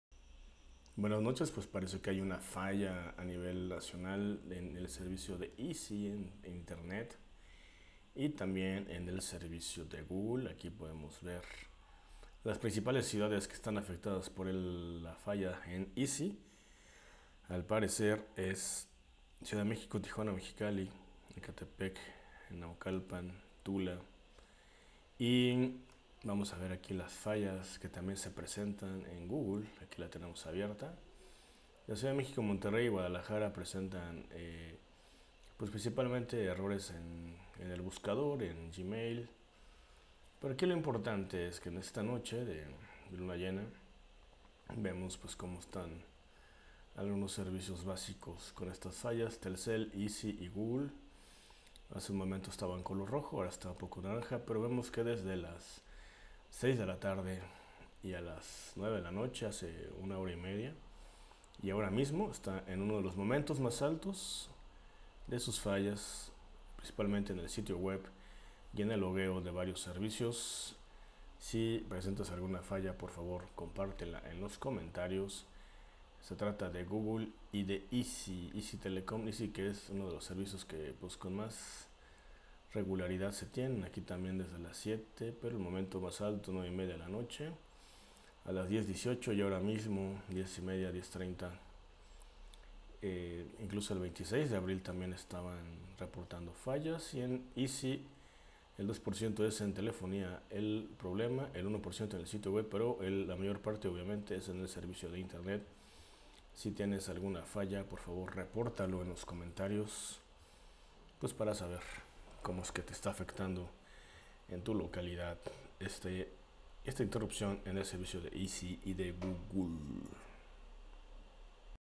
Falla en Izzi y Google 27 de Abril de 2021
Se reporta vía Downdetector.mx  una importante falla en los servicios de Internet de Izzi y en Google. Si tienes algun problema, repórtale en los comentarios